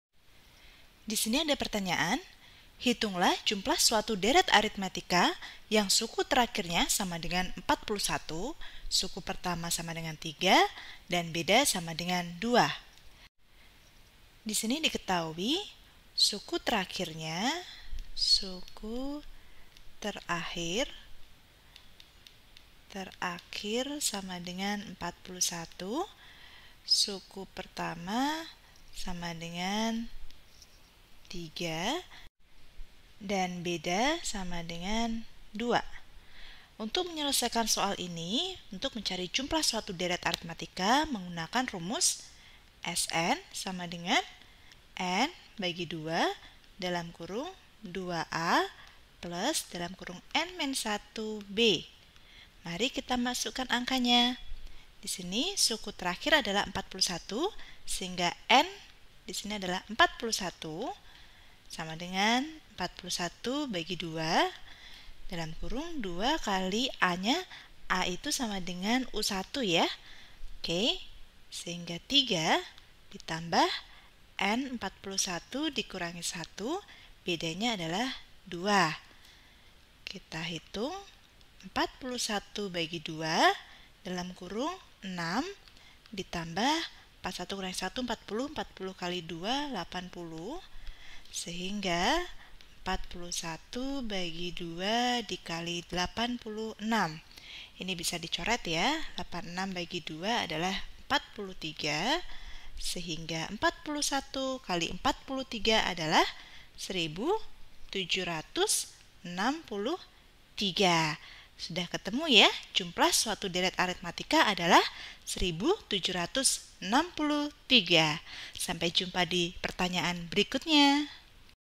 Hitunglah jumlah suatu deret aritmetika yang suku terakhirnya = 41, suku pertama = 3, dan beda 21 =
Topik atau Materi: Deret Aritmetika - POLA BILANGAN DAN BARISAN BILANGAN - BILANGAN - Matematika Kelas 8 SMP - K-13

Sumber Soal: Matematika 3B kelas 9 K-13, MCA 2013 , Erlangga

00:00 : Bedah Soal
01:00 : Konsep, rumus dan pengertian Deret Aritmetika
01:18 : Langkah penyelesaian soal
01:01 : Jawaban akhir
01:31 : Penutup